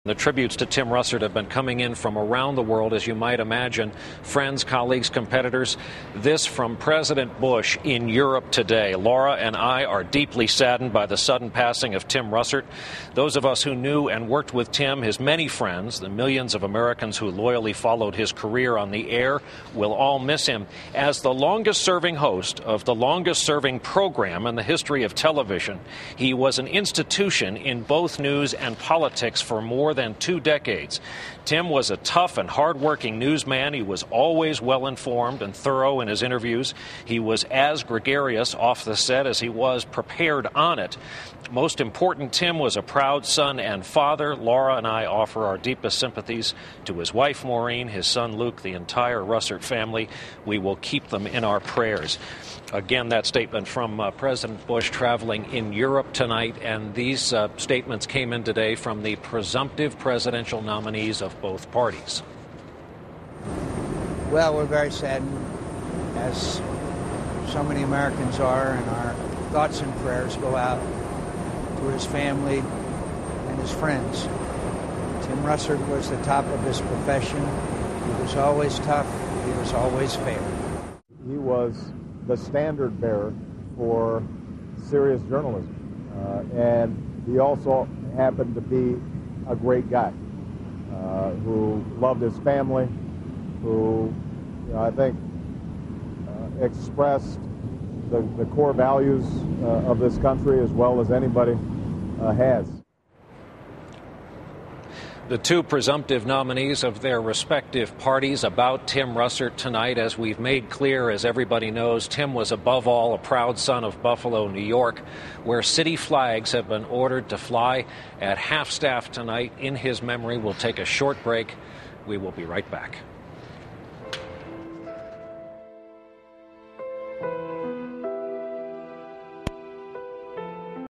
TIM RUSSERT McCain Obama pay tribute 6-13-08
Tim Russert died Friday afternoon of a heart attack while in the newsroom at NBC headquarters in Washington, DC. He died doing the thing he loved the most- readying himself for another edition of Sunday morning's Meet the Press.

Video copyright NBC, MSNBC